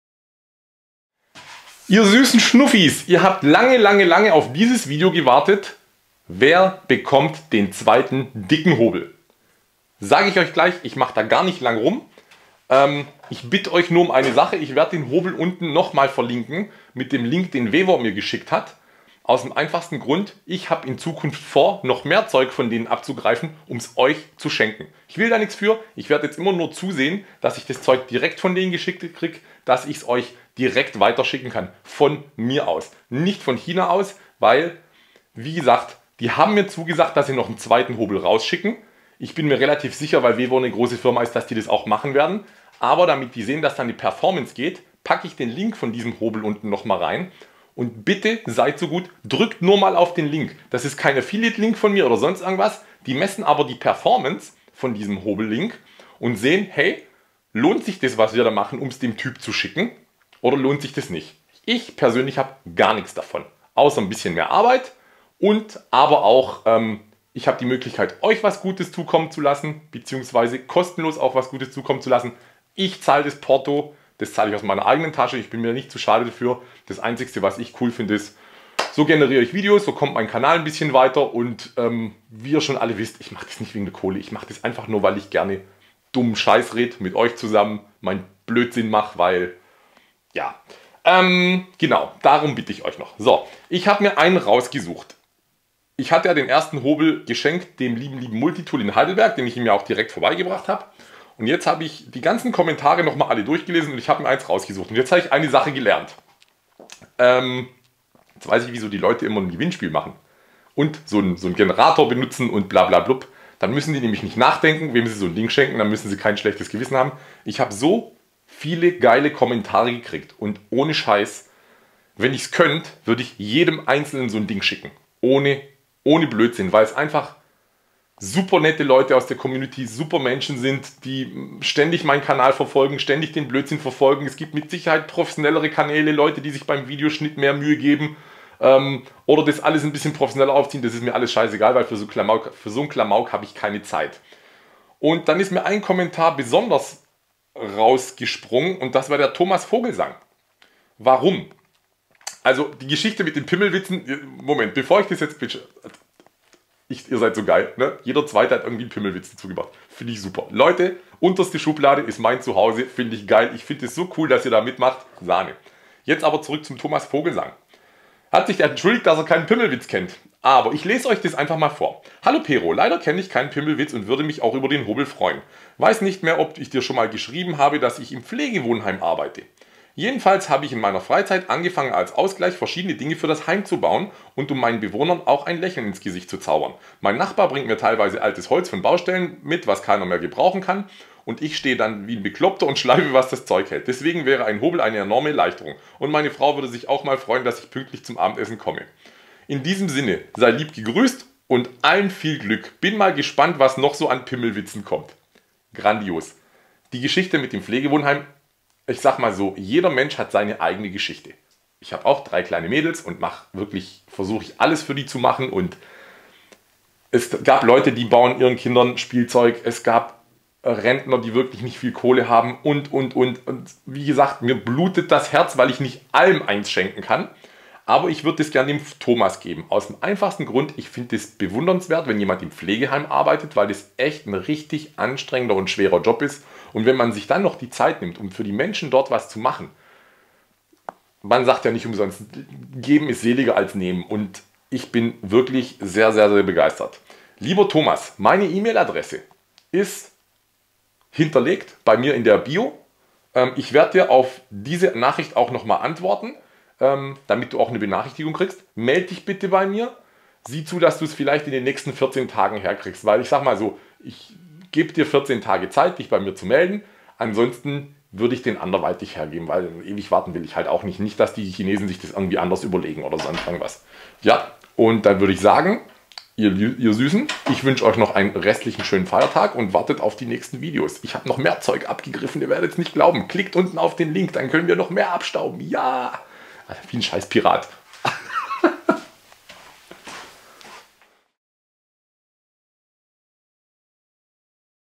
Wer hat den 2. Vevor Dickenhobel bekommen? erfahrt es hier!
Das ist der Besagte Dickenhobel:
SSHK83S6

Endlich schaffe ich es zeitlich den 2. Hobel raus zu hauen!
Ich leite den Versand dann ein zu dir! :-)

Bleibt mir treu und habt spass!